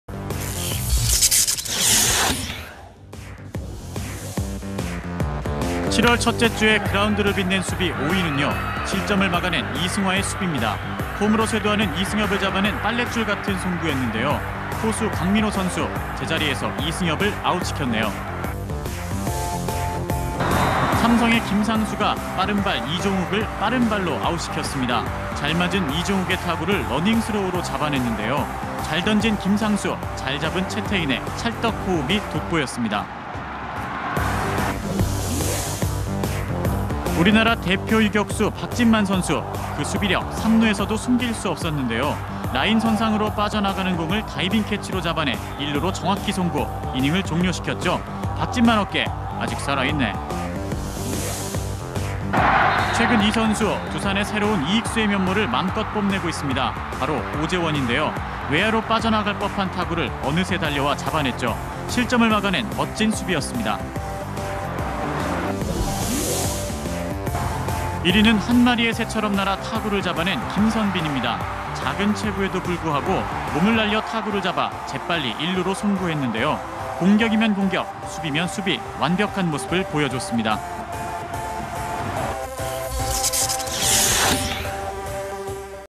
[7월 1주차] ADT캡스플레이 TOP5
금주의 ADT캡스플레이 TOP5

 1위) 한 마리 새처럼 날아 타구를 잡아낸 "KIA 김선빈"
2위) 외야로 빠져나가는 공을 다이빙캐치로 잡아낸 "두산 오재원"
3위) 라인선상으로 빠져나가는 공을 다이빙캐치로 잡아낸 "SK 박진만
4위) 빠른발 두산 이종욱을 러닝스로우로 아웃시킨 "삼성 김상수"
5위) 홈으로 쇄도하는 삼성 이승엽선수를 잡기 위한 빨래줄 송구를 보여준 "롯데 이승화"